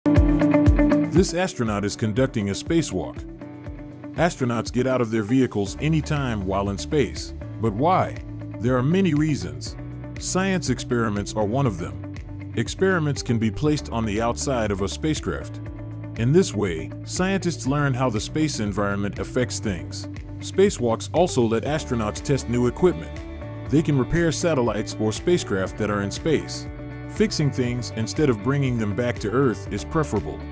Why astronauts going on spacewalks
This astronaut is conducting a spacewalk. Astronauts get out of their vehicles any time while in space. But why? There are many reasons. Science experiments are one of them.